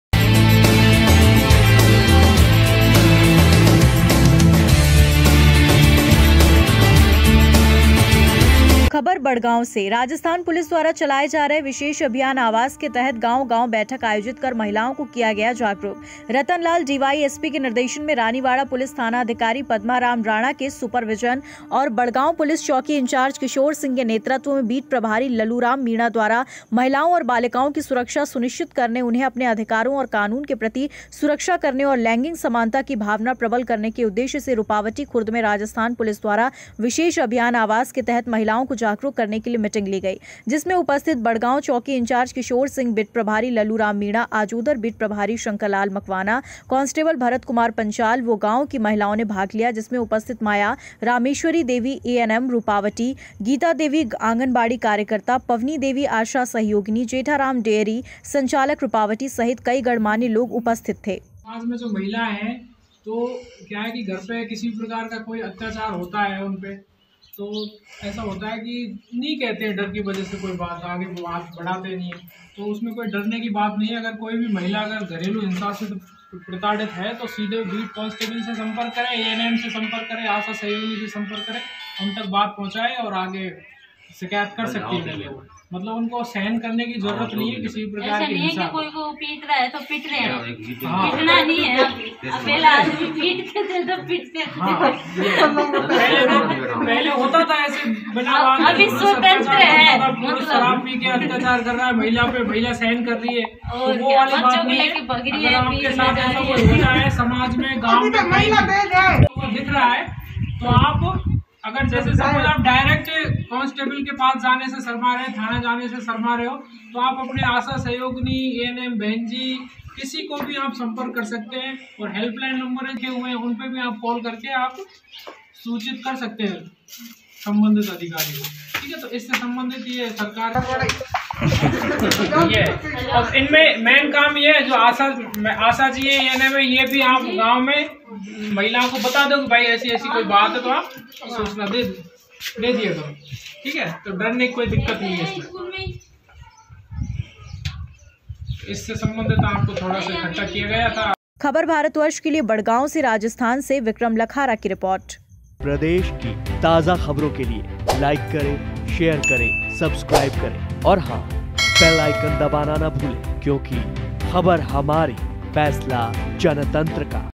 बडगांव (राजस्थान) : अभियान आवाज के तहत बैठक आयोजित कर महिलाओं को जागरूक किया
सुपरविजन व बडगांव पुलिस चौकी इंचार्ज किशोर सिंह के नेर्तत्व में कार्यक्रम
 रूपावटी खुर्द में राजस्थान पुलिस द्वारा विशेष अभियान आवाज के तहत बैठक
           महिलाओं व बालिकाओं की सुरक्षा सुनिश्चित करने उन्हें अपने अधिकारों की जानकारी दी